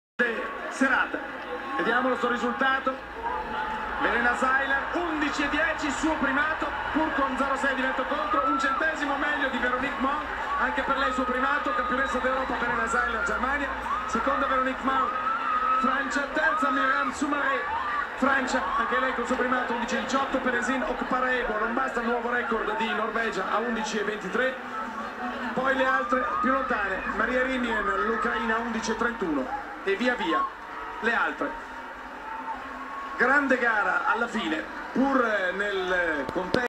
Veronique Mang e Myriam Soumarè isteriche.mp4
Veronique Mang e Myriam Soumarè isteriche dopo l'argento e il bronzo nei 100 metri femminili ai campionati europei di atletica leggera di Barcellona 2010